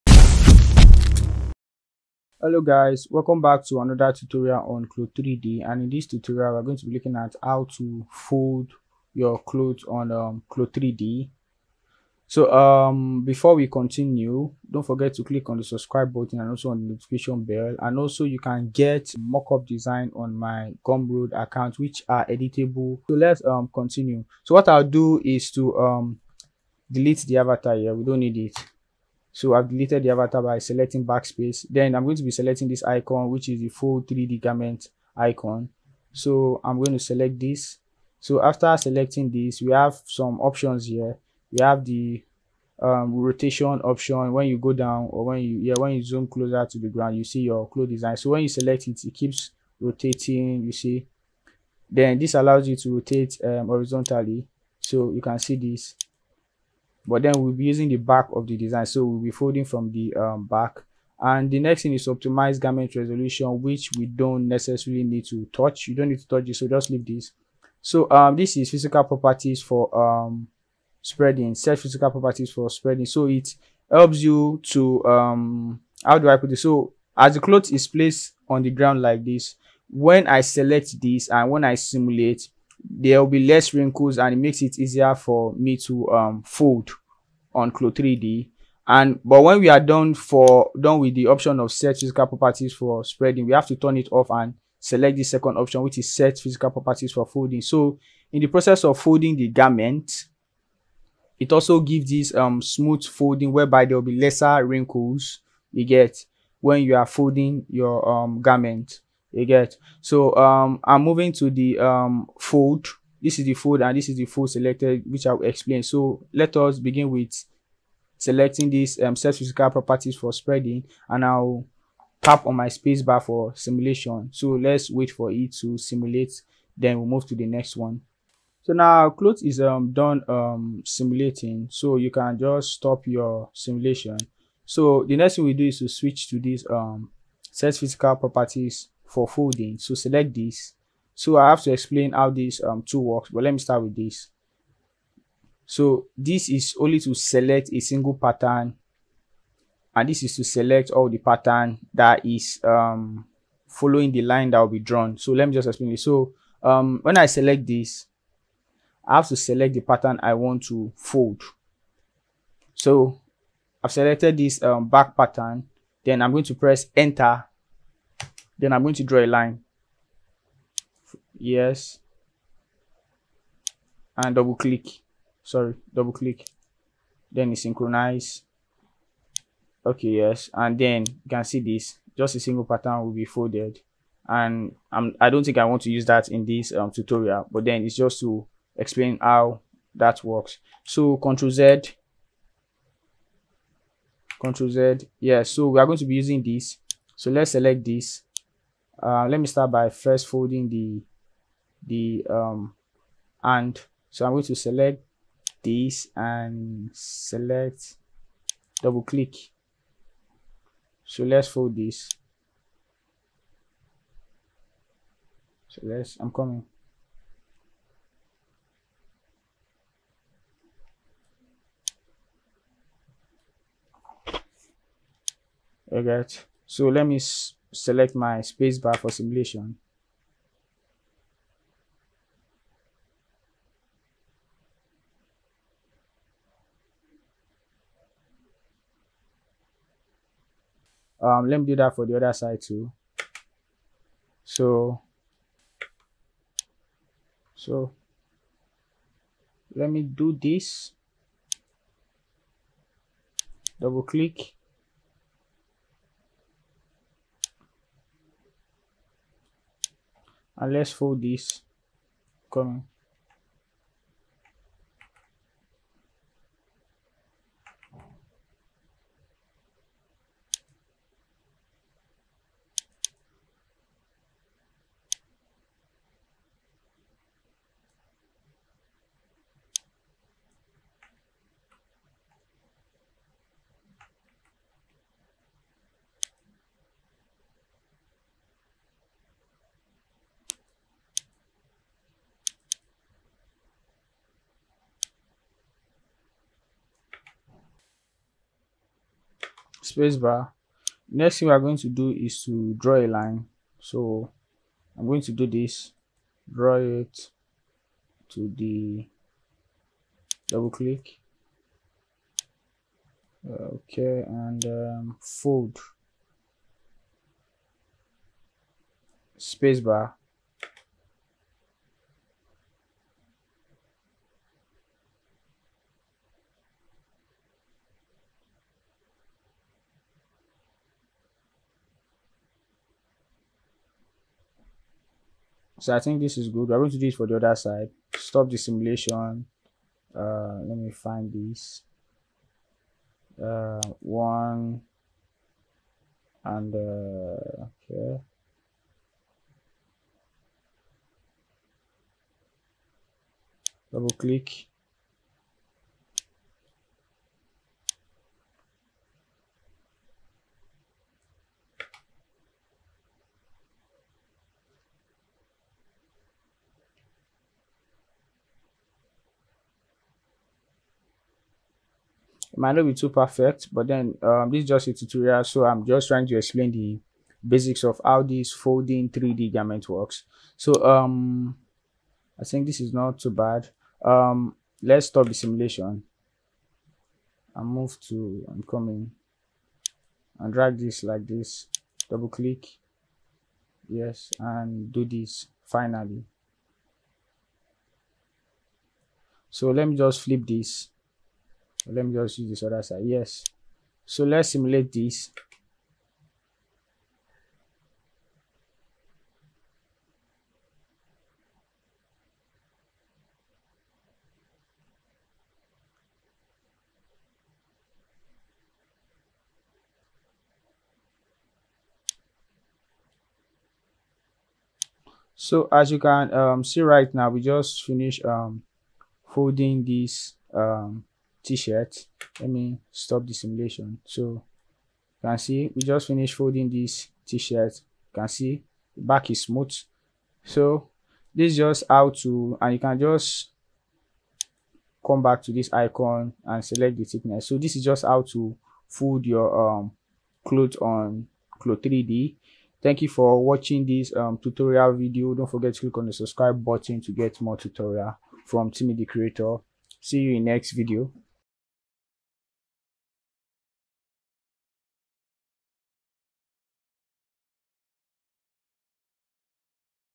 Clo3d Tutorial: How to fold Clothes or T-shirt Mockup
In the Clo3D tutorial on folding clothes on a T-shirt, you start by opening the software and loading your T-shirt model. Using the folding tool, draw fold lines on the T-shirt where you want the folds to appear. Adjust the angles of these lines to achieve the desired look, tweaking the fold depth and sharpness for realism. Run a simulation to see how the folds behave, making any necessary adjustments to ensure they look natural. Once satisfied, save your project and export the T-shirt model for further use or rendering.